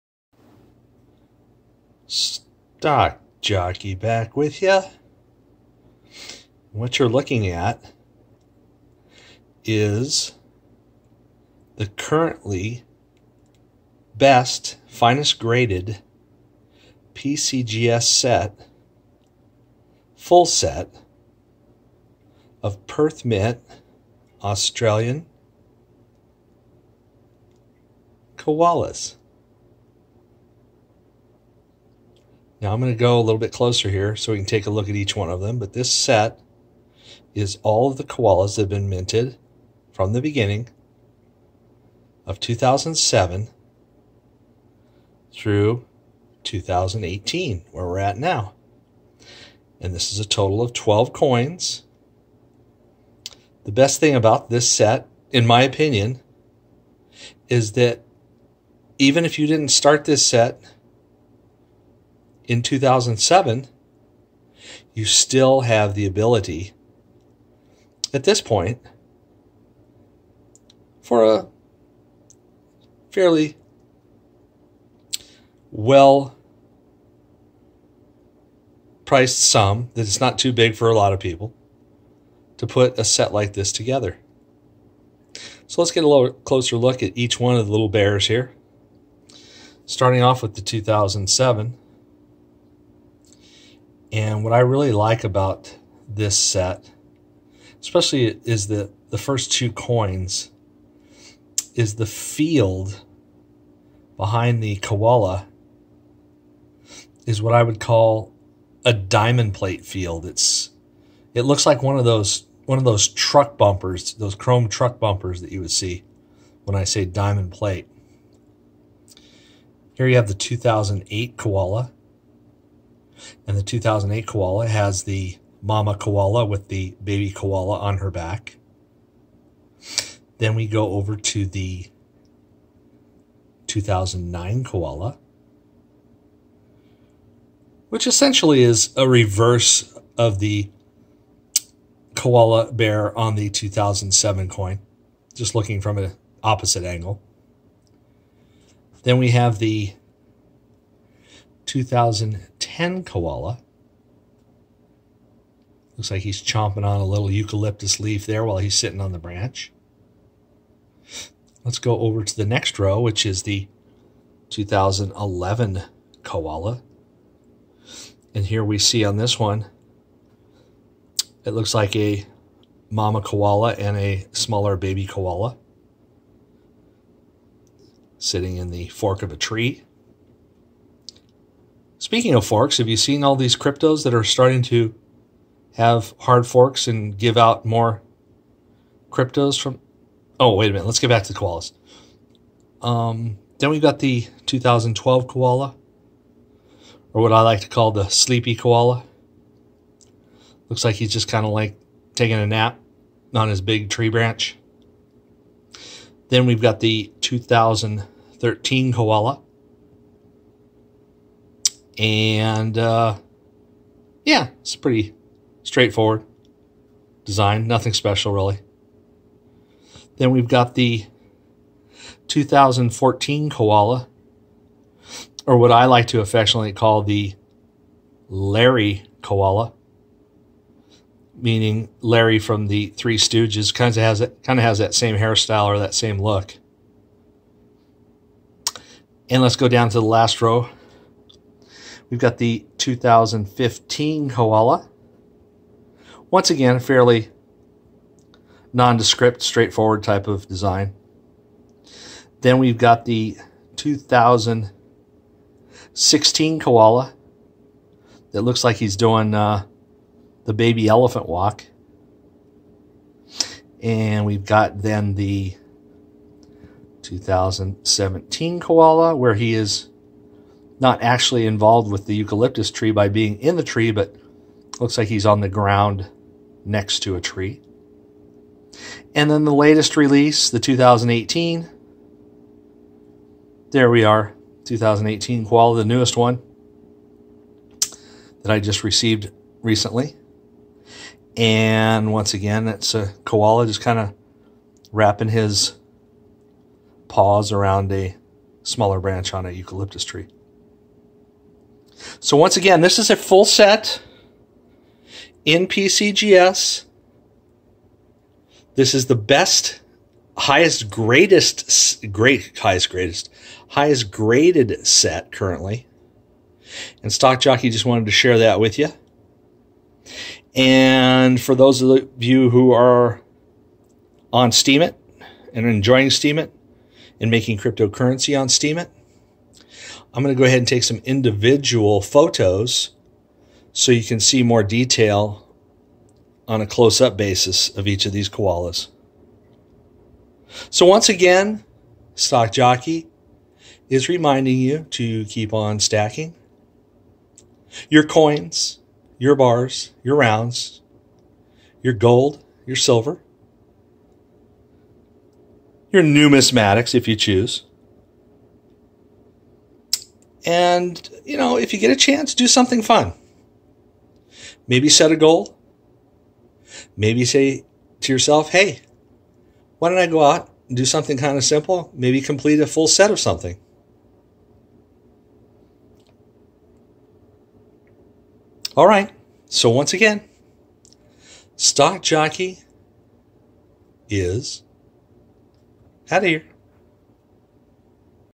Complete Set of Perth Mint Silver Koala's 2007-2018
This is the currently Finest Graded Set of Silver Koala's on PCGS.
Enjoy individual Pictures of each coin also on STEEMIT. Follow me also @stokjockey.